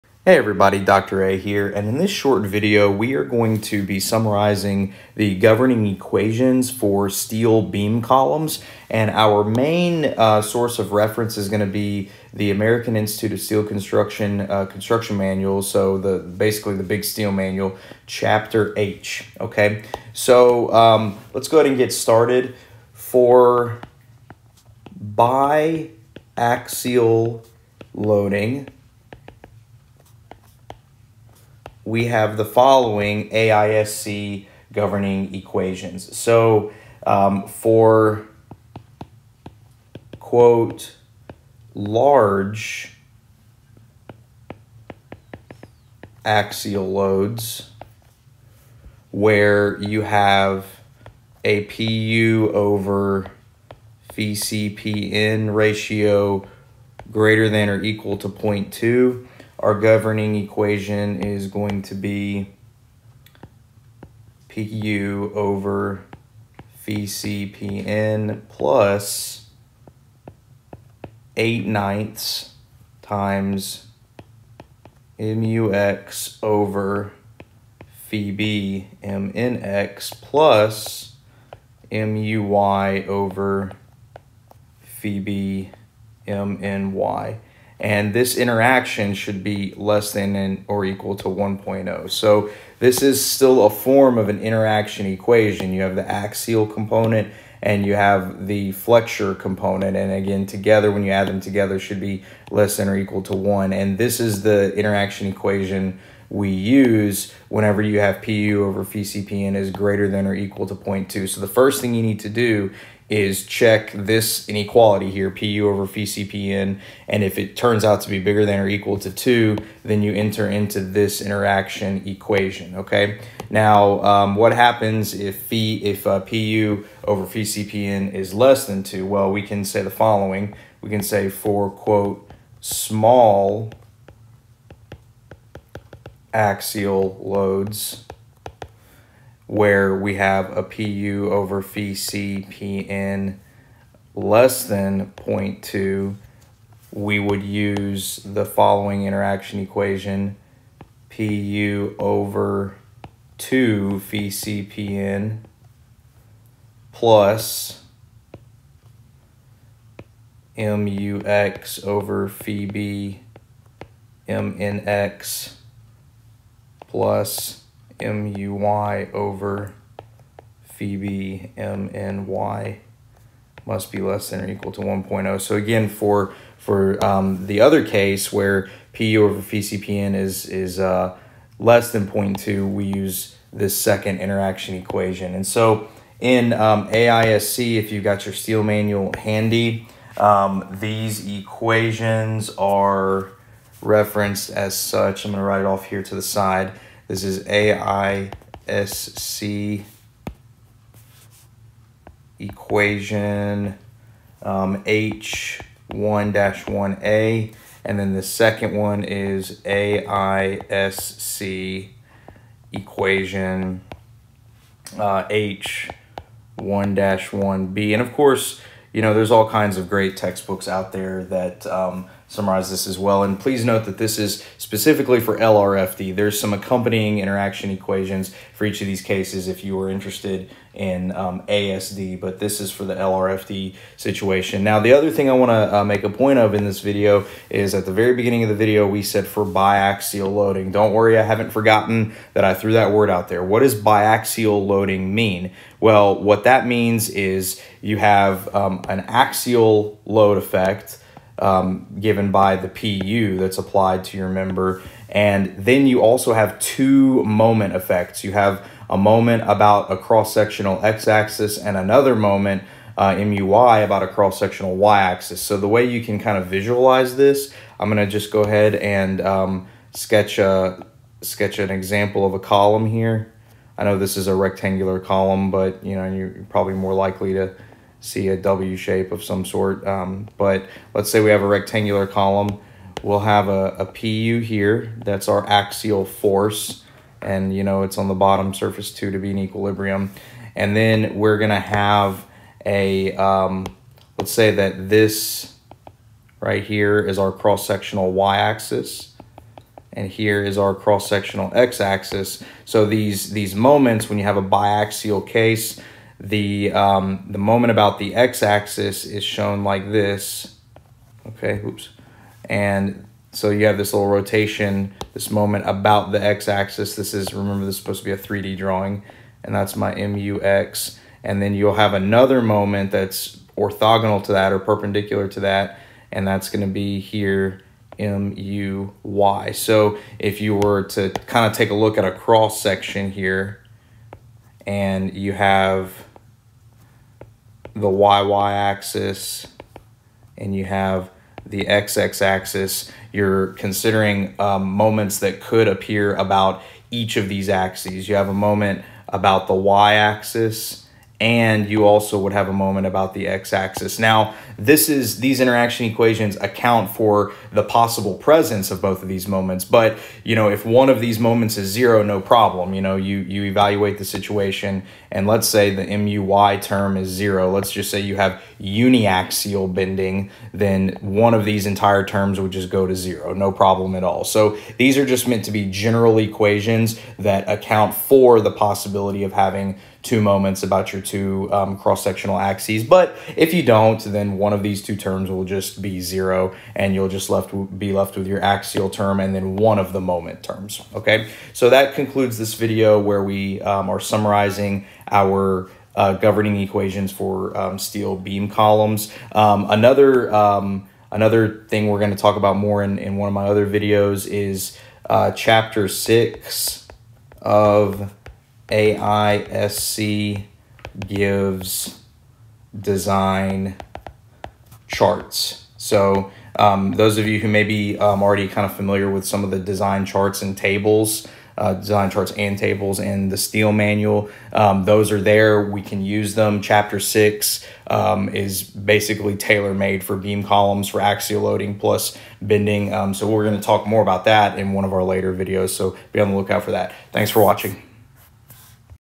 Steel Beam-Column Background (Doubly and Singly Symmetric Sections)
Steel Design, Beam-Column, AISC, Steel, Interaction Equations:

In this video we discuss some background information regarding steel beam-column using AISC interaction equations.  NOTE THAT THESE EQUATIONS ARE VALID FOR DOUBLY AND SINGLY SYMMETRIC SECTIONS.